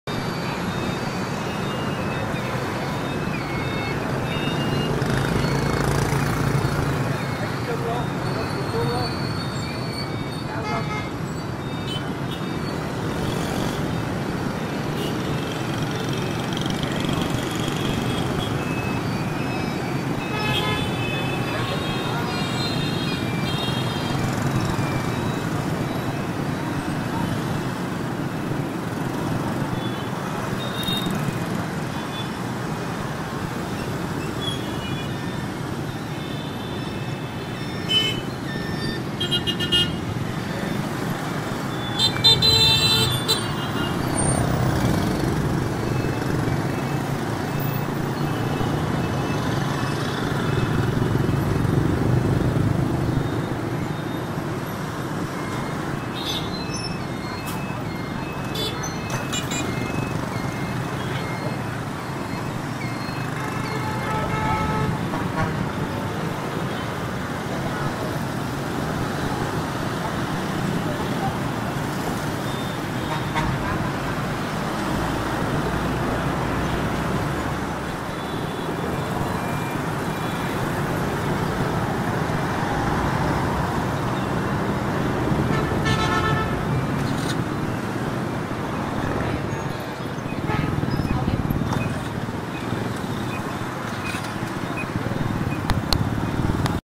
Rất nhộn nhịp
các bạn sem lúc công nhân đi làm tại Bình Dương